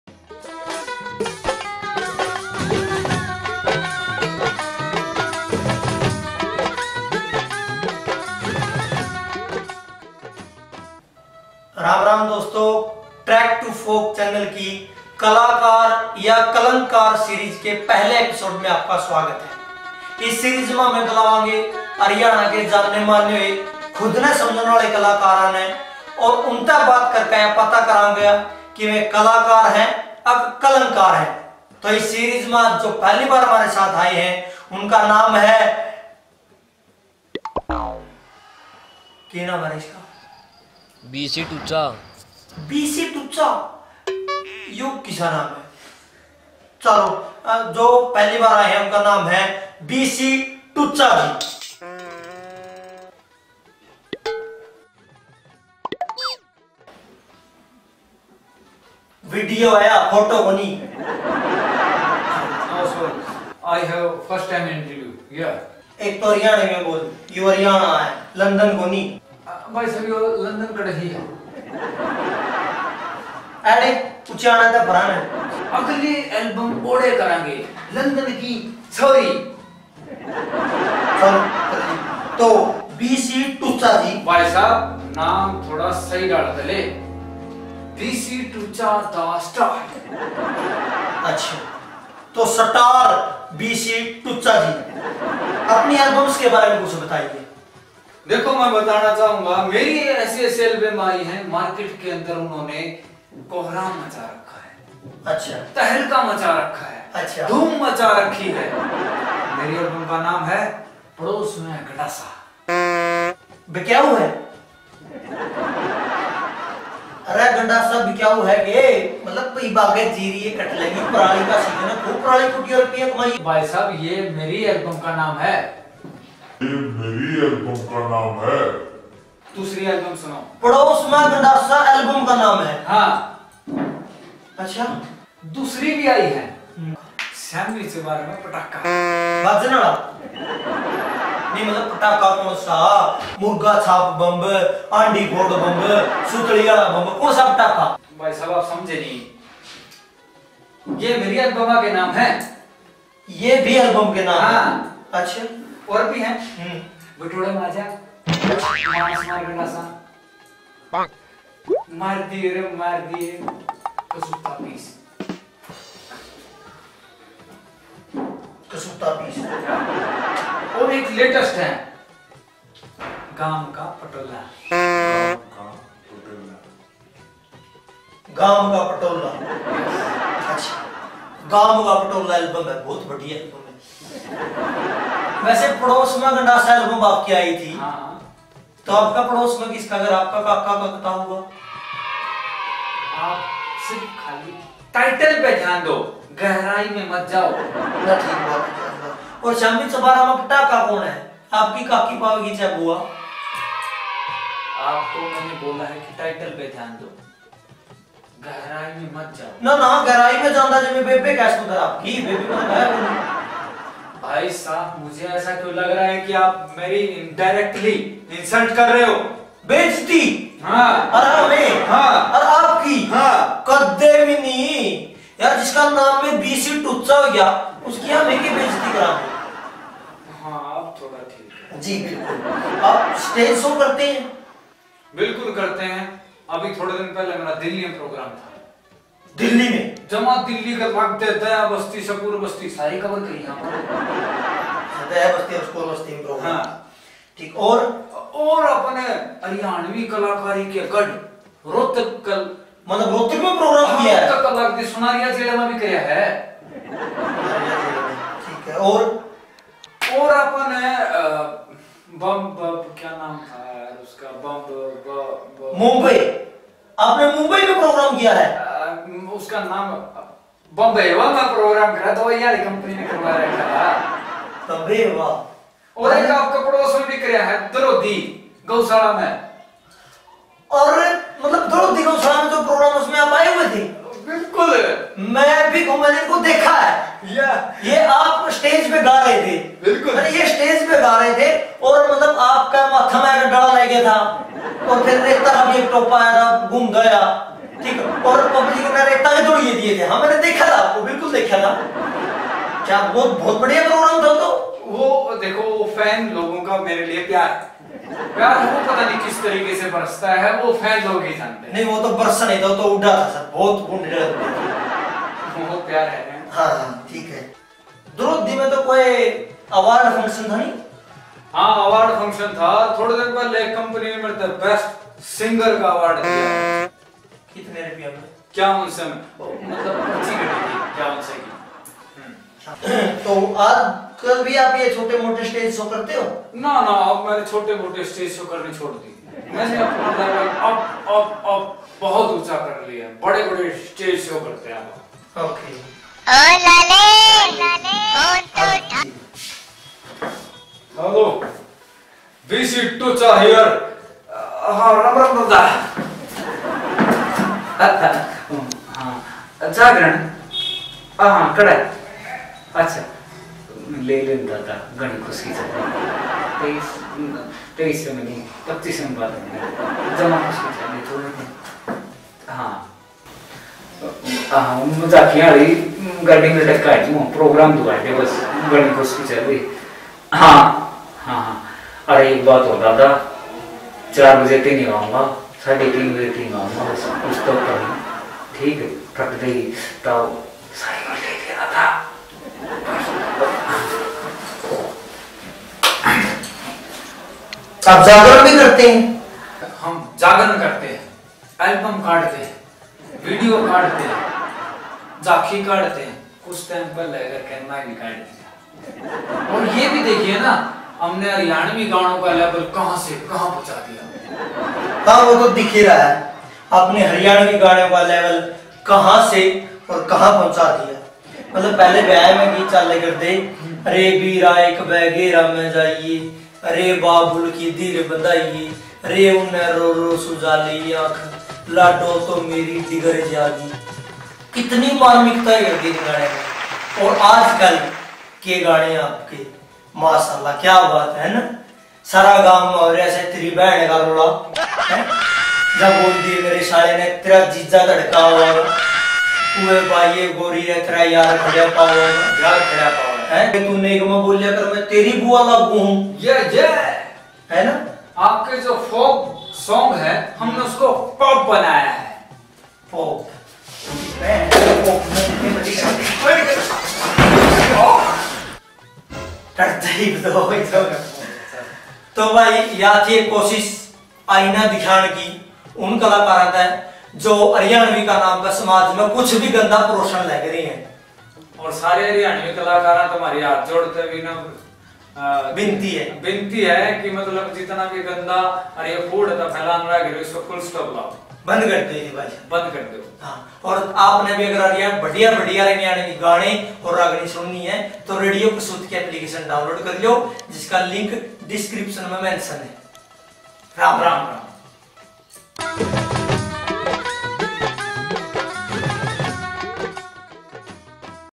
हरियाणवी सिंगर का इंटरव्यू .....कलाकार या कलंक कार ........पार्ट 1 by trek to folk
हरियाणवी सिंगर का इंटरव्यू .कलाकार या कलंक कार पार्ट 1 ये एक कोशिश है आईना दिखाने की उन कलाकारों को जो हरियाणवी के नाम पर समाज में कुछ भी कचरा परोस रहे हैं। ... radio kasoot app .